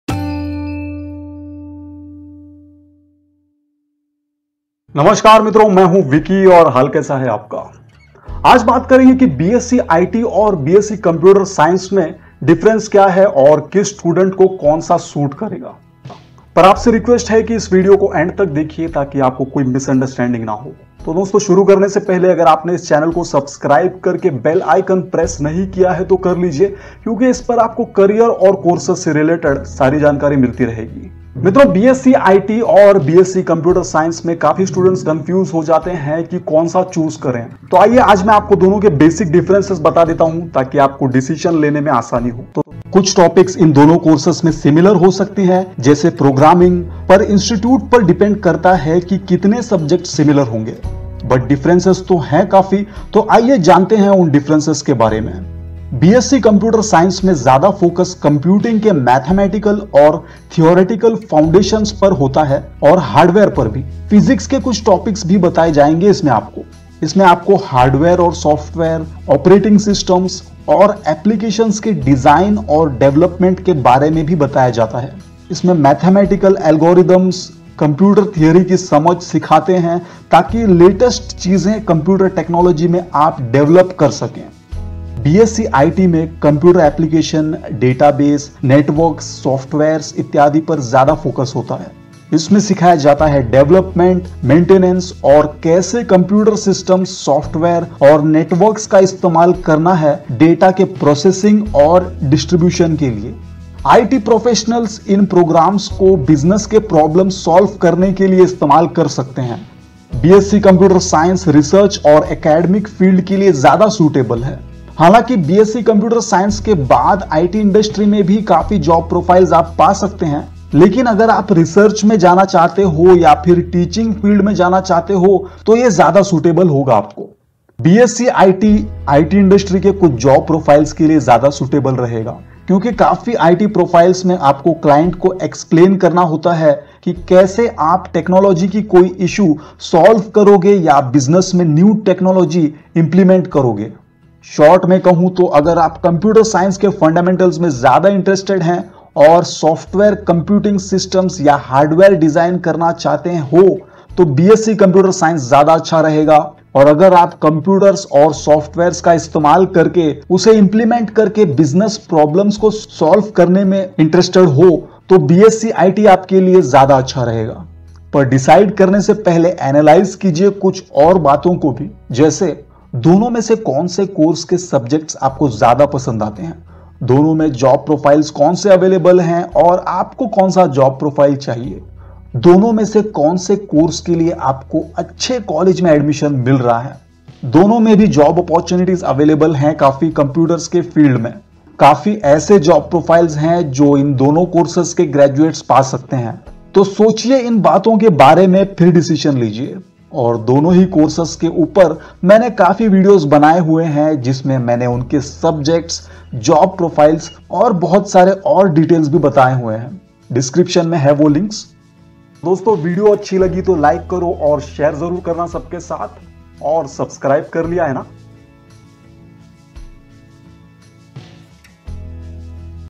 B.Sc. IT vs B.Sc. Computer Science | Difference kya hai? BSc IT or BSc CS which is better?
B.Sc. IT vs B.Sc. Computer Science. Difference between B.Sc Information Technology and B.Sc Computer Science. BSc it or BSc cs which is better?